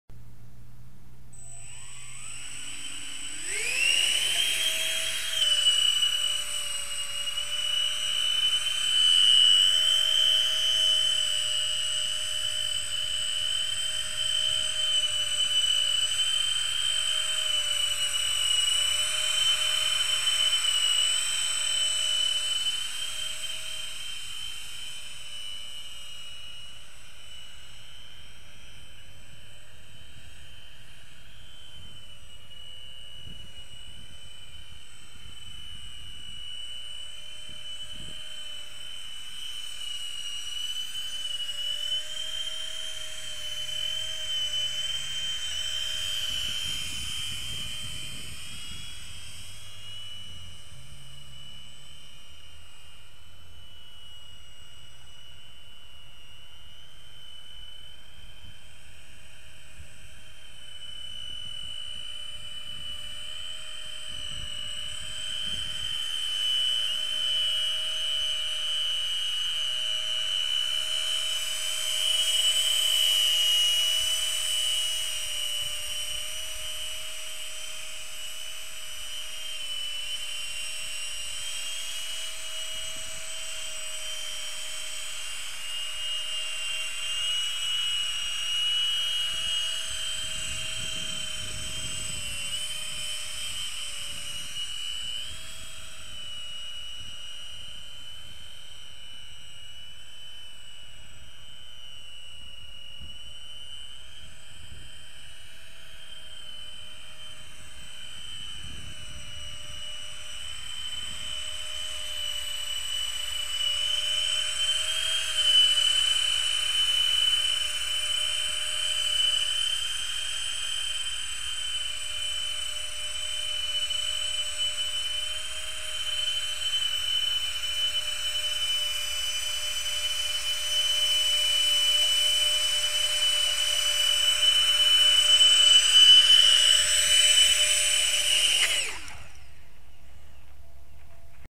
THRUST VECTORING
A flight to test the response to extended servo ranges. More responsive and very stable. Awesome cam platform...